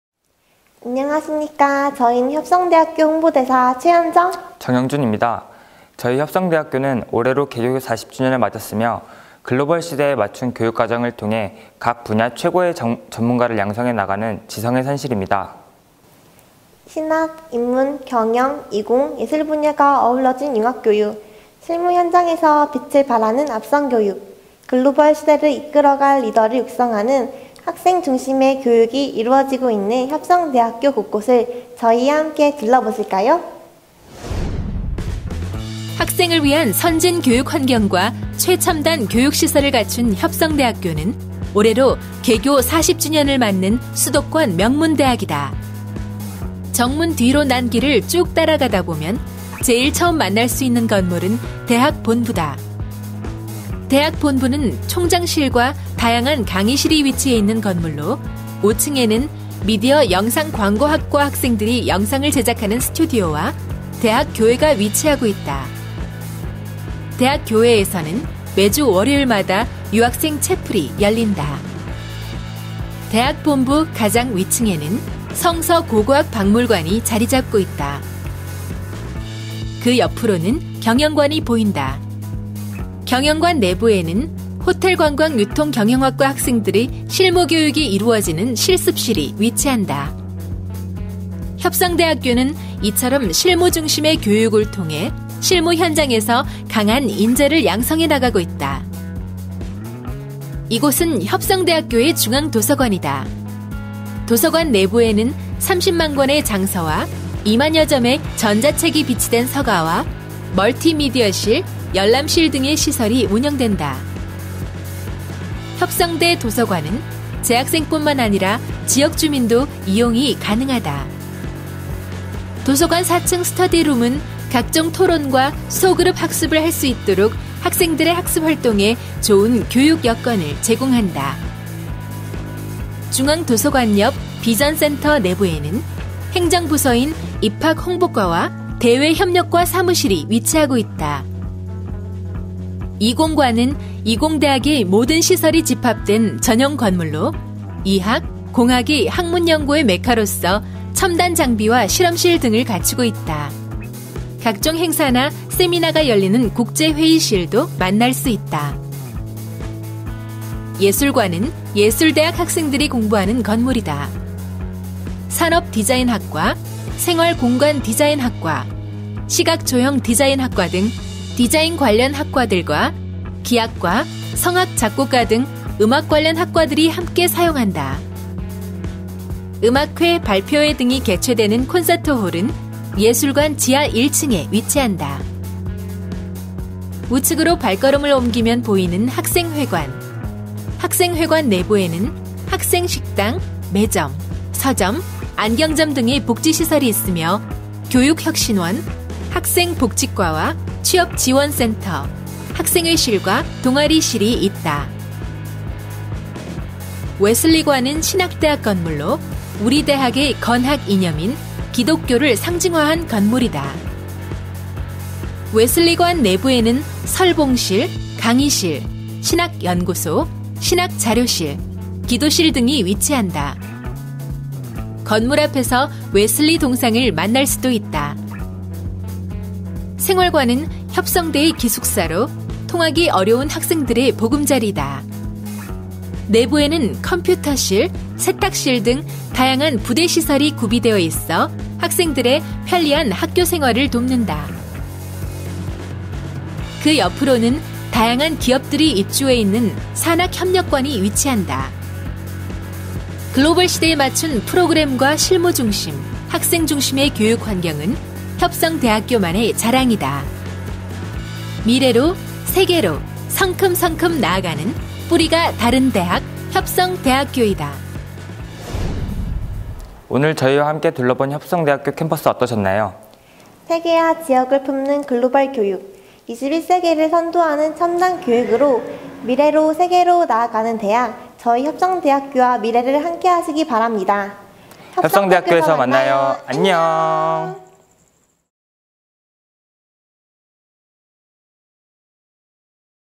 [하이틴TV]협성대학교_미리가 본 대학
글로벌시대의 맞춘 교육과정과 최고의 전문가를 양성, 실무현장에서 빛을 바라는 앞선 교육을 하는 협성대학교!
실무중심, 학생중심의 교육환경을 자랑하는 미리가보는 협성대학교!
지금부터 보실까요?